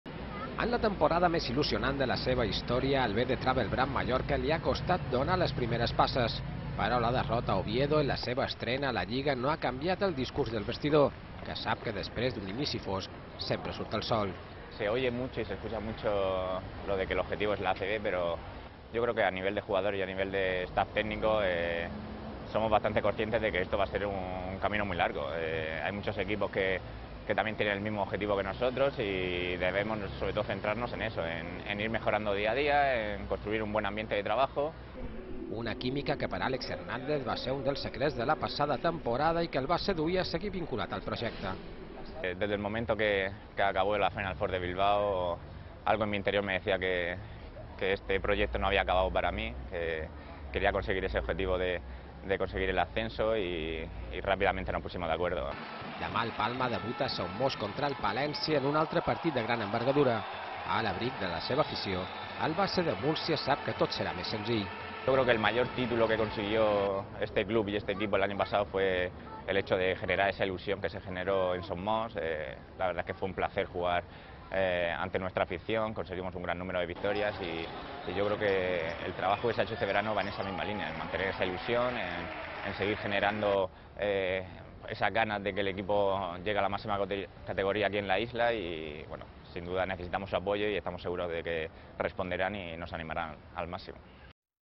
Álex Hernández, la brúixola del B the travel brand Mallorca
Álex Hernández és un dels jugadors més importants de l’equip de Félix Alonso. El base espera una bona estrena del Palma davant la seva afició.